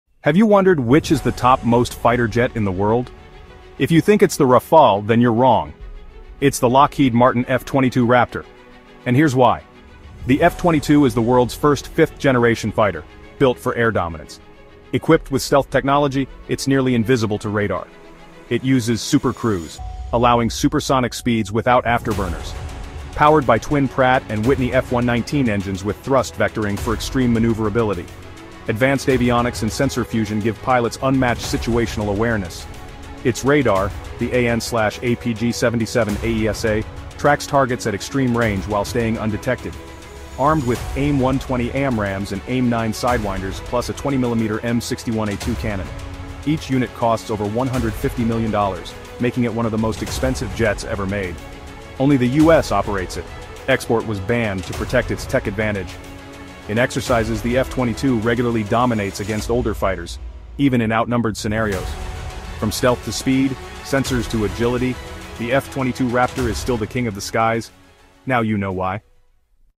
F-22 Raptor: King of the Skies!
✈️ F-22 Raptor: The Ultimate Air Superiority Fighter

Discover the unmatched capabilities of the F-22 Raptor, the U.S. Air Force's premier fifth-generation stealth fighter. From its supercruise ability to its advanced avionics and stealth design, the F-22 sets the standard for modern aerial combat. In this video, we delve into its design, performance, and the technology that keeps it at the forefront of air dominance.